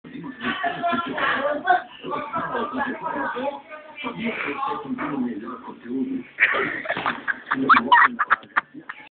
Português.3gp
serginho tentendo ficar em pé em cima do galão de água